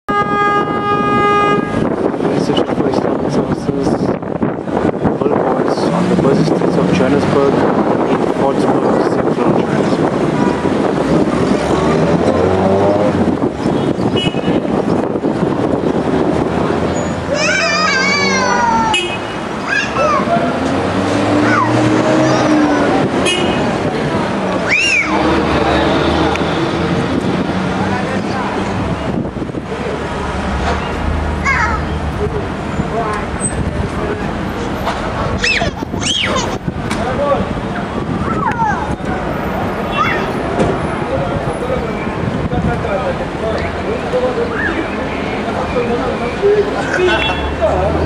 Islam on billboards - Dawah in Fordsburg Square - Johannesburg 2 of 2
A video showing the billboards on Islam for the purpose of Dawah (inviting to Islam) erected by the Institute for Islamic Services at Fordsburg Square in Johannesburg part 2.